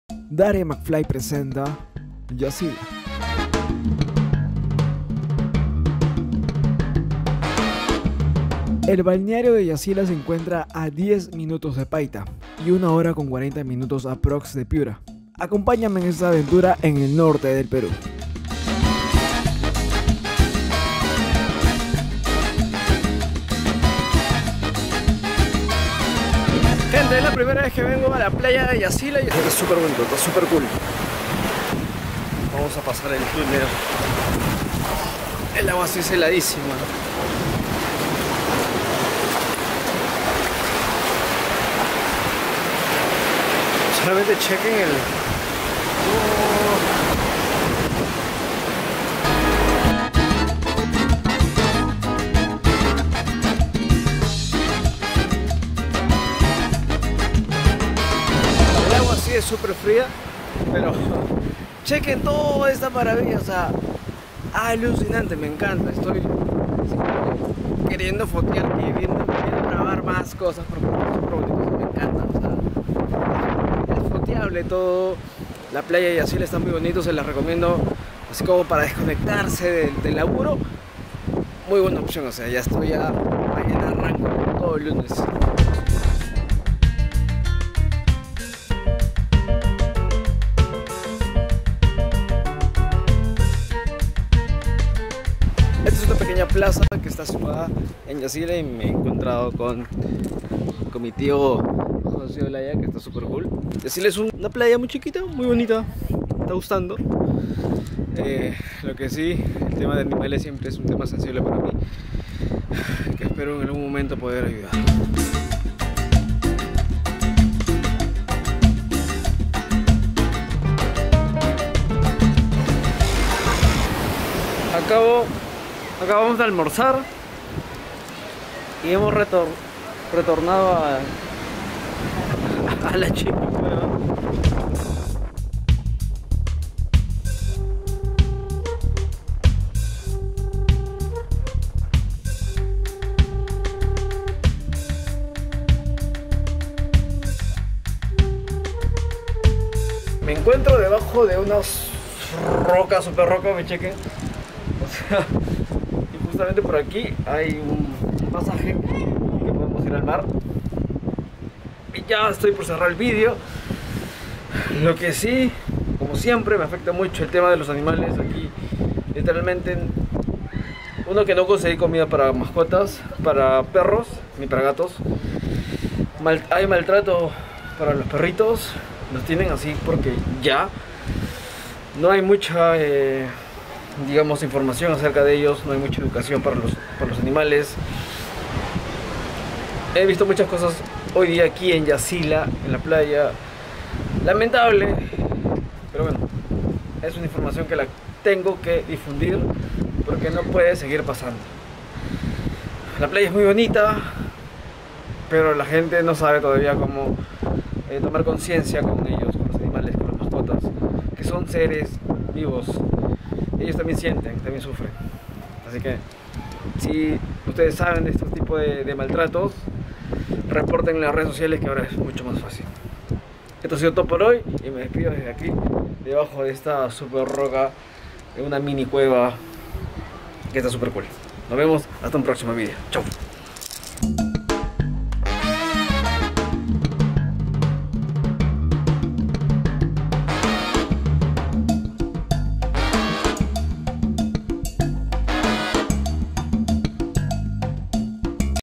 Yacila | Una playa cerca a Paita - Piura
Un viaje corto a Yacila, espero volver, me gustó mucho pero el tema de los animales me choca mucho, no tolero el maltrato.
Disfruten la naturaleza, y si saben de algún maltrato animal, hagan la denuncia en redes, nosotros podemos parar ello.